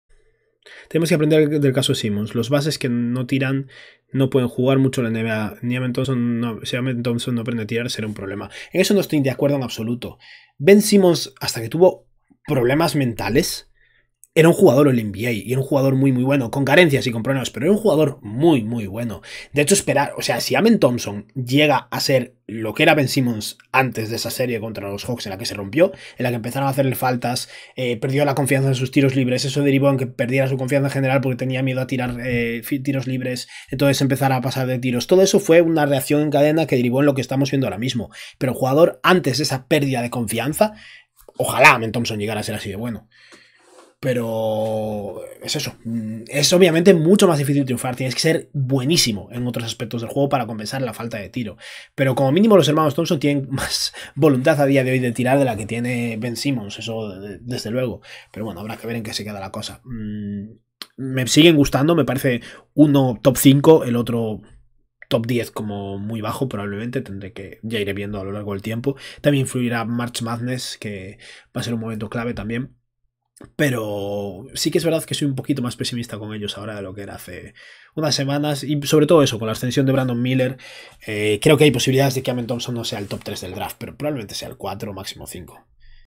Amen Thompson podría llegar a funcionar sin un tiro | BASKET 365
Clip en el que daré mi opinión sobre la idea de que un base incapaz de tirar no puede funcionar en la NBA.